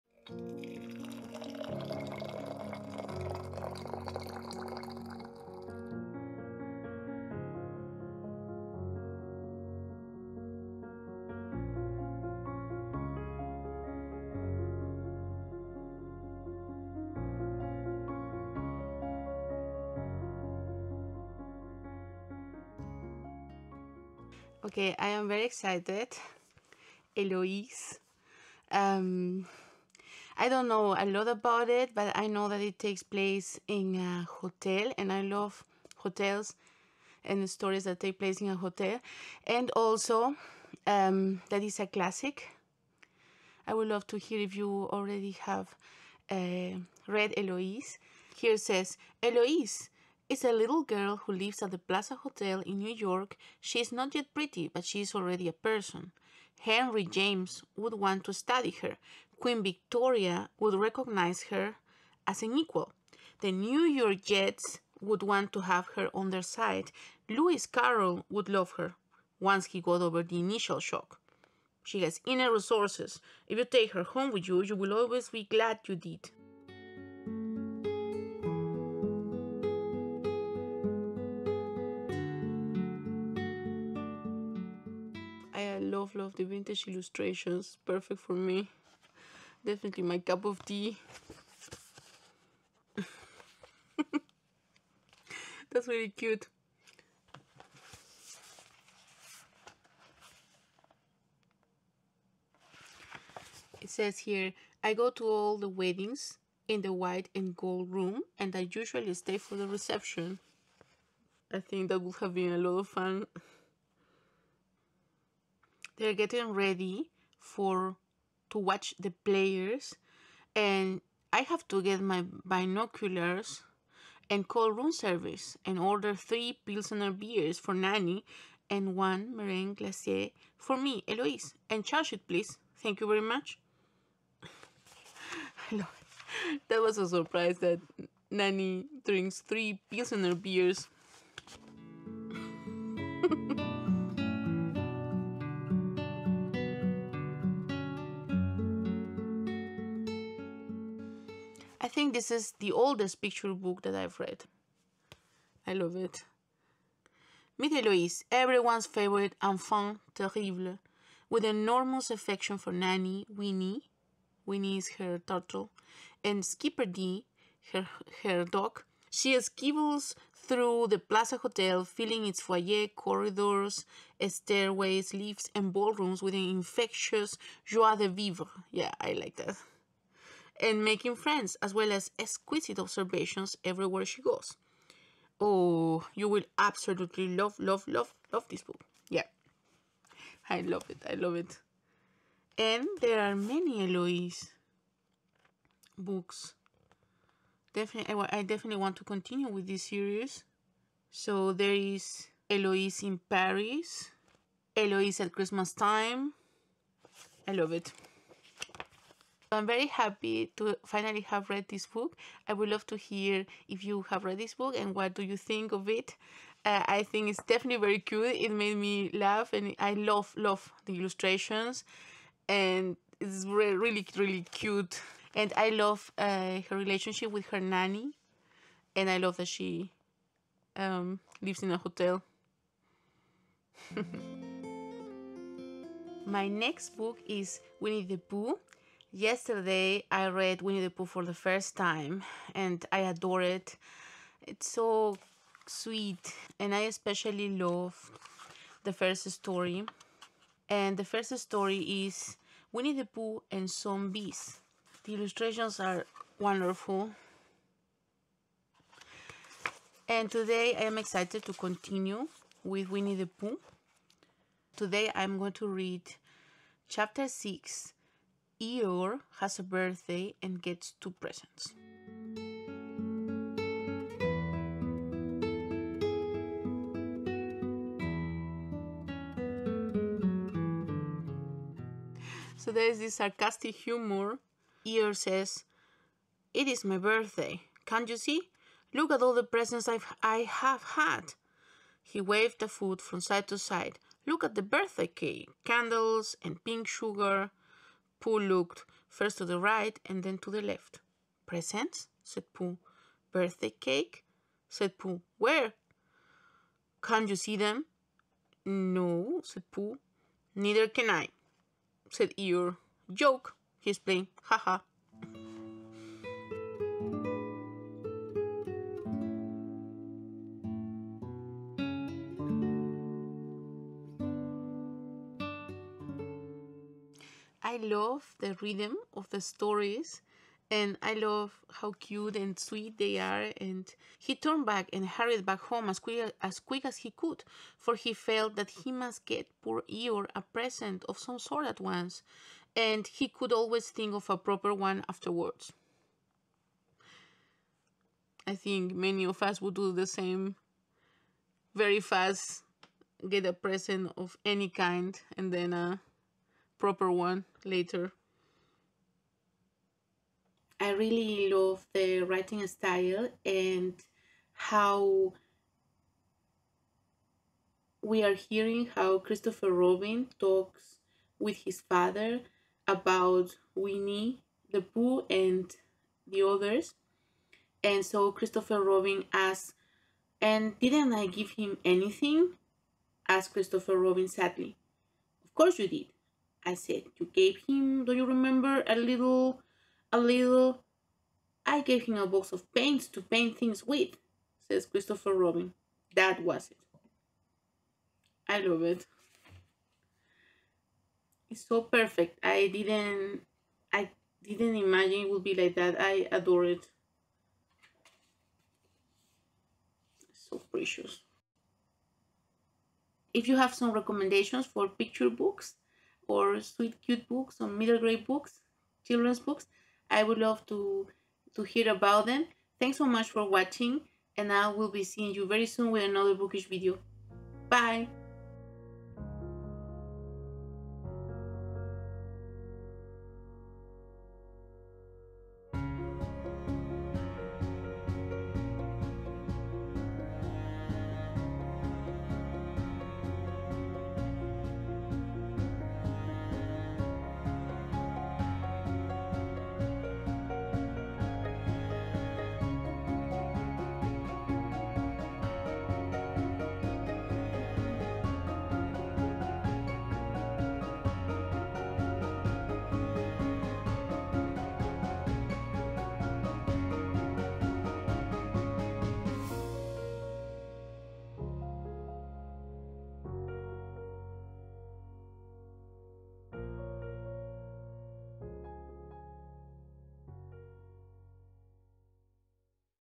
Reading My Favorite Cute Books | Enchanted Spring💐
Cozy Afternoon Reading VLOG. Reading my favorite cute books this May.